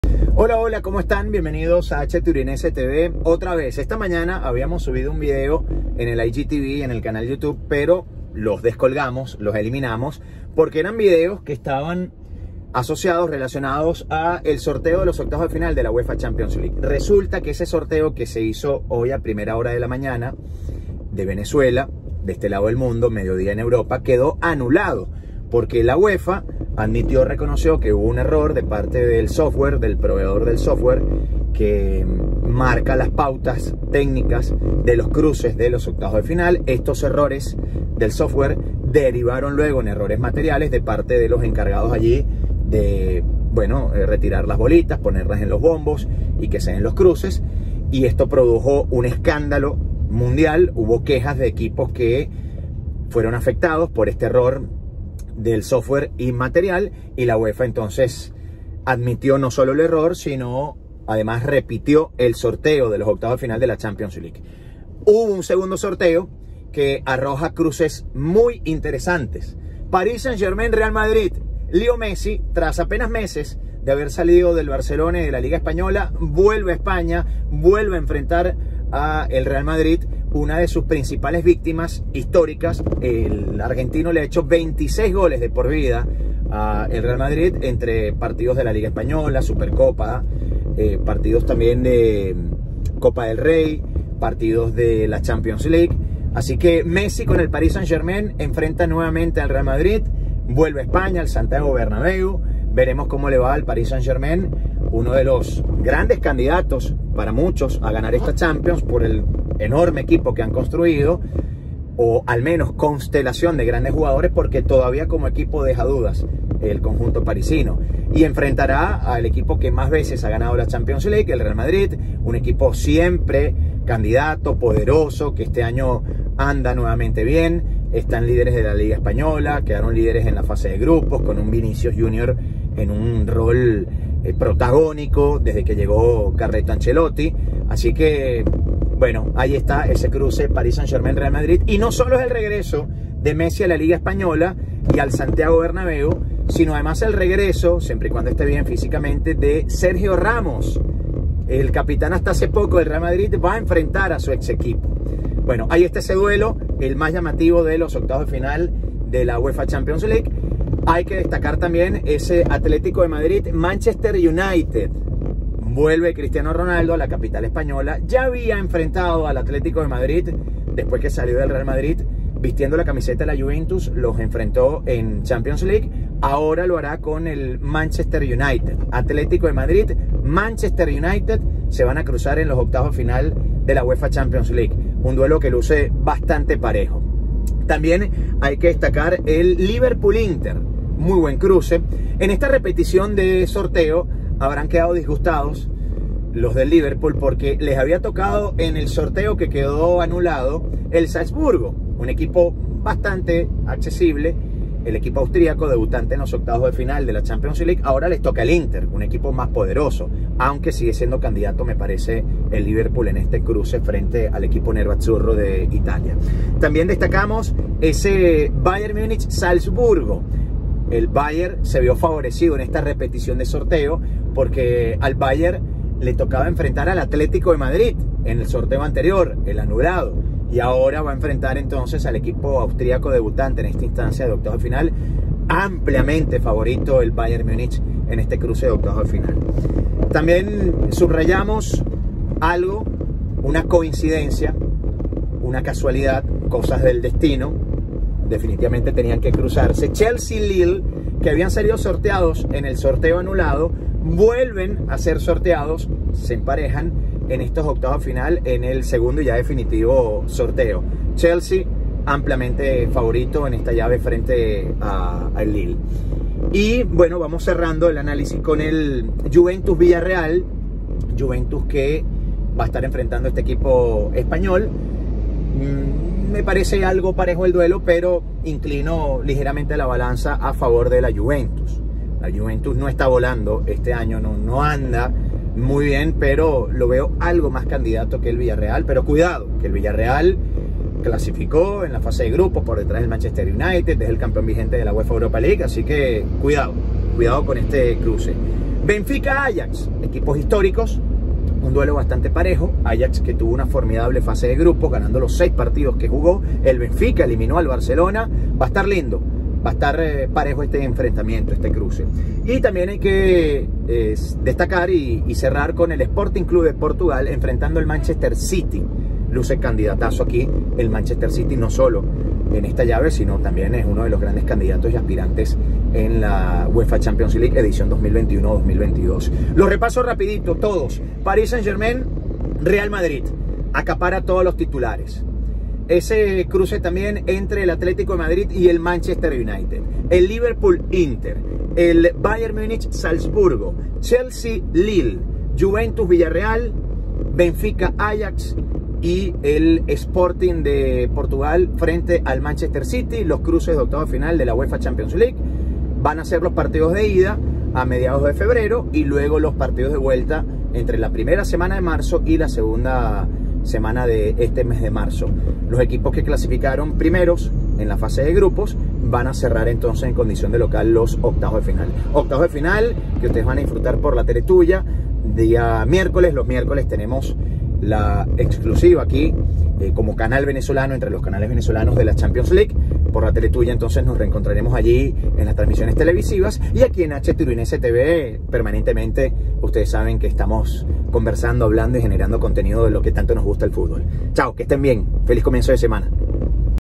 CHAMPIONS LEAGUE: MESSI Y RAMOS VOLVERÁN Al BERNABÉU
En HTURINESETV analizamos TODOS los cruces de 8vos. de FINAL de la Champions League 2021/2022 en el sorteo celebrado este lunes 13 de diciembre. En un primer sorteo hubo errores técnicos y materiales anunciados por la UEFA. Se tuvo que realizar de nuevo y así quedaron los cruces, siendo el más atractivo el PSG vs. Real Madrid.